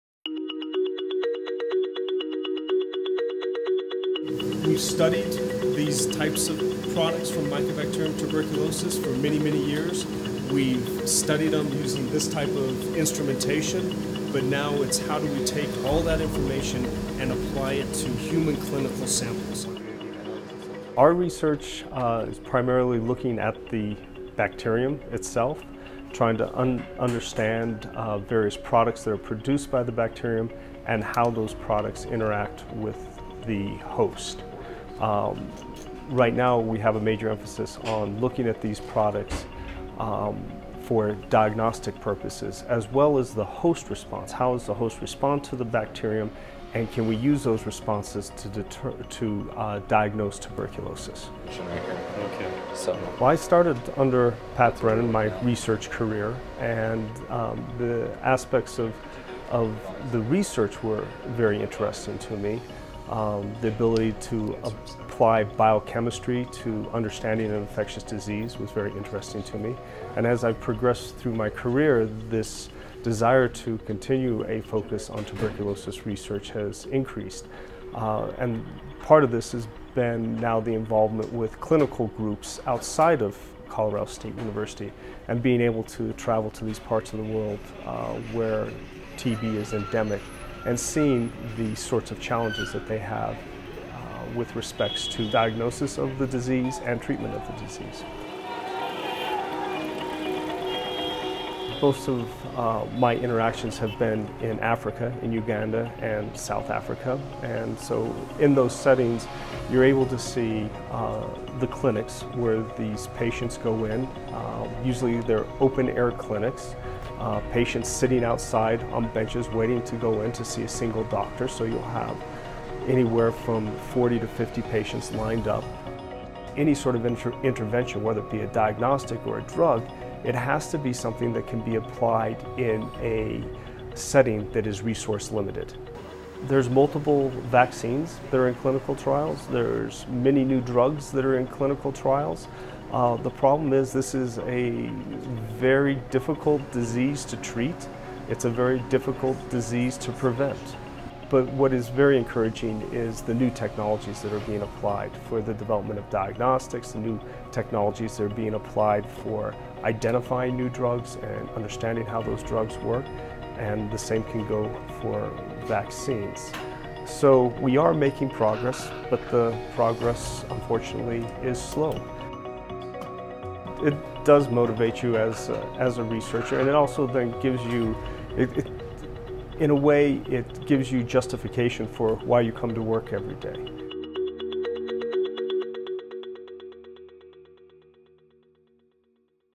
Colorado State University researcher John Belisle describes bacterium that causes tuberculosis
John Belisle, a researcher at Colorado State University, discusses the challenges of finding a way to cure tuberculosis, including limited resources in countries plagued by TB, as well as the complexities of research going into better understand the disease.